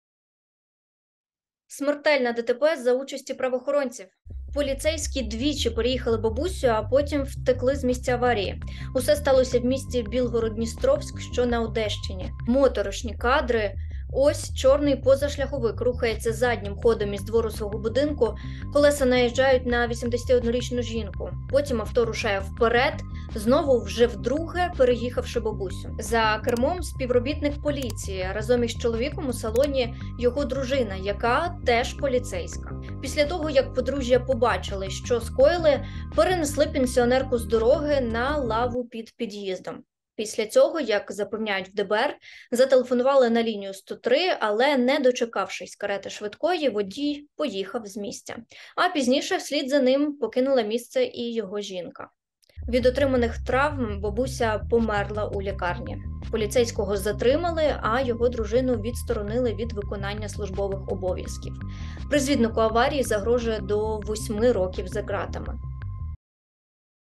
Поліцейські двічі переїхали бабусю на Одещині і втекли з місця ДТП
Смертельна ДТП за участі правохоронців. Поліцейські двічі переїхали бабусю, а потім втекли з місця аварії.
Усе сталося в місті Білгород-Дністровськ, що на Одещині. Моторошні кадри. Відео не рекомендовано до перегляду людям зі слабкою психікою.